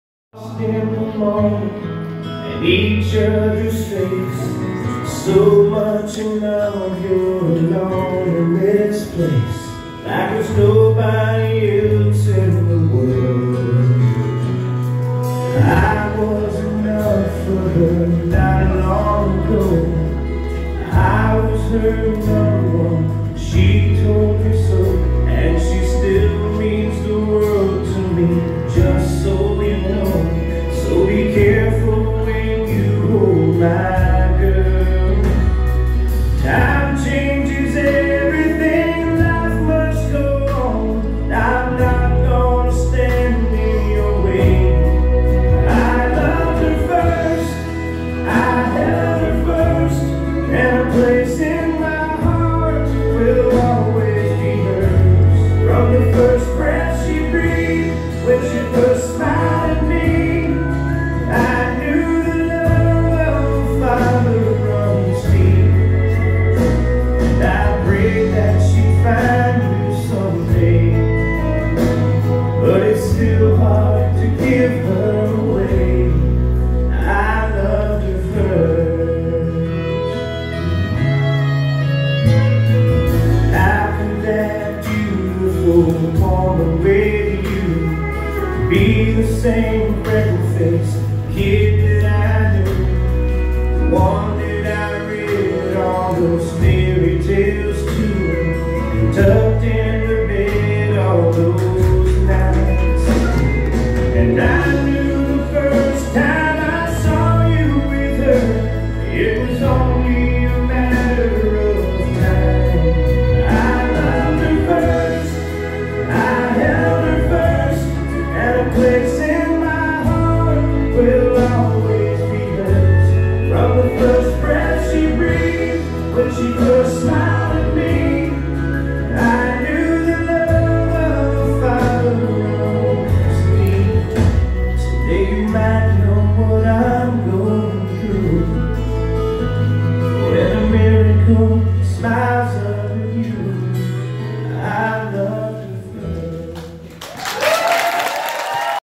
Daddy-Daughter Dance - Erika Beams-Martin and Jim Martin
No question this is the best Daddy-Daughter dance I've seen ... so happy to have been there!  And congratulations to Arthur and Erika!